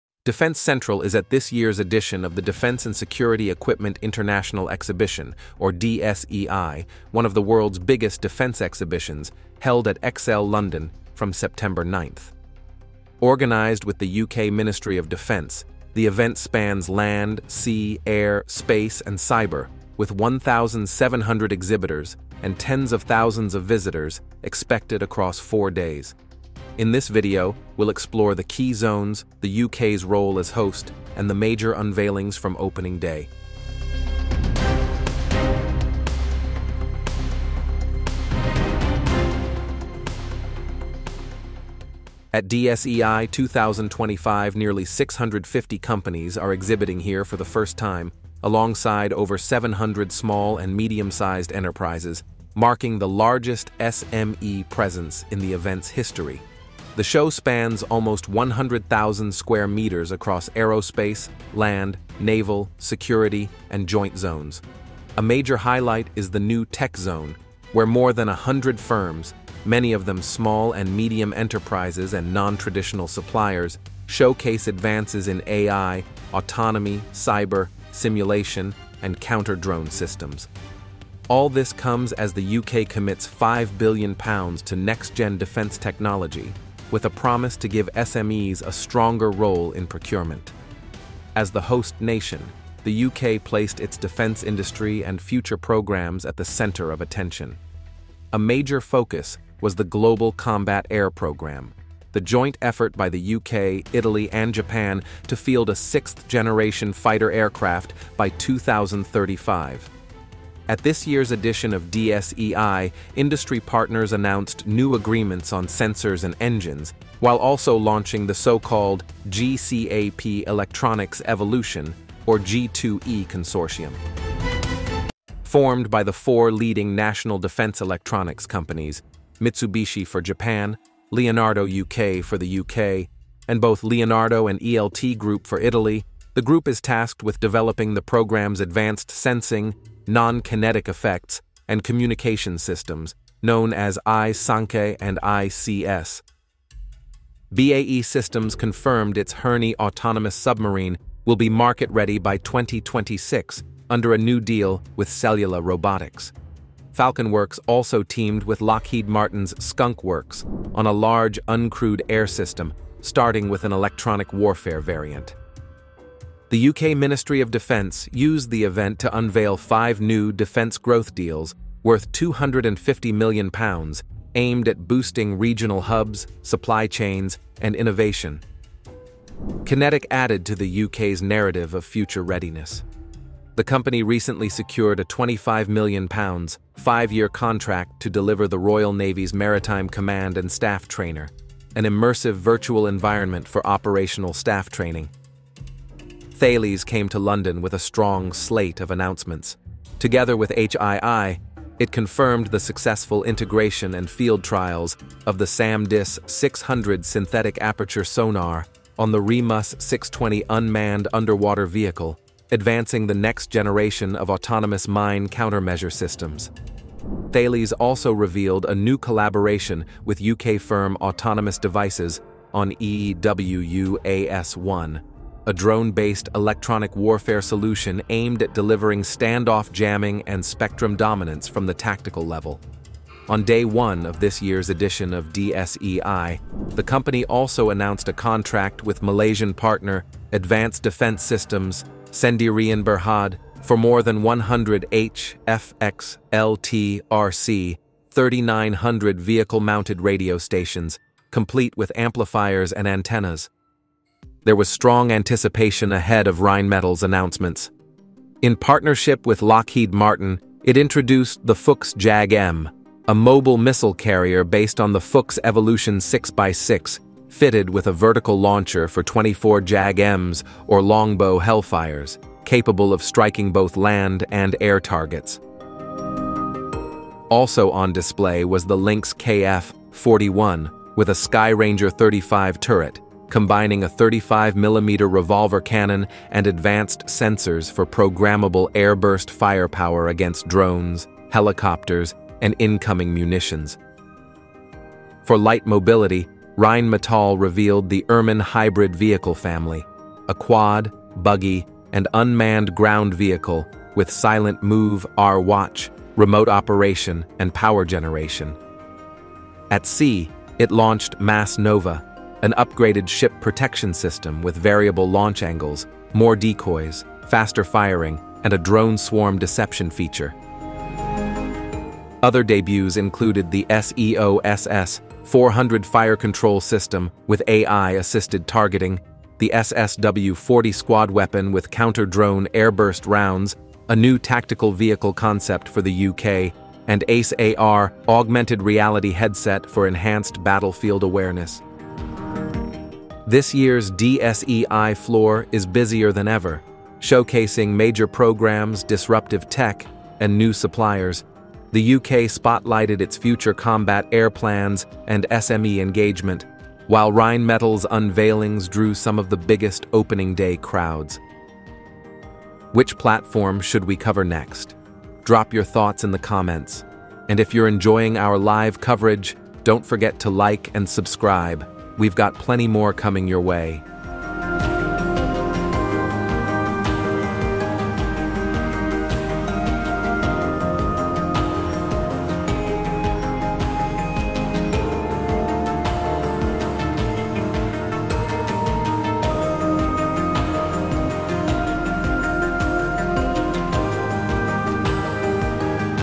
DSEI 2025: Global Combat Air Programme, Fuchs JAGM & AI Systems – Tech Unveiled
At DSEI 2025, London’s massive defence and security exhibition, over 1,700 companies showcased next-generation military technology spanning land, sea, air, space, and cyber domains. Highlights included the UK’s Global Combat Air Programme progress, BAE Systems’ Herne autonomous submarine, Rheinmetall’s Fuchs JAGM missile carrier, Lynx KF41 with Skyranger 35 turret, and advanced unmanned vehicles and drone countermeasure systems. The new Tech Zone emphasized AI, autonomy, electronic warfare, and simulation technologies, particularly from SMEs, reflecting the UK’s £5 billion investment in future defence. Innovations like QinetiQ’s immersive MCAST trainer, Thales’ EW-UAS1 drones, and MASS nova naval protection system demonstrated how modern defence is increasingly networked, autonomous, and modular, offering both high-tech lethality and operational adaptability.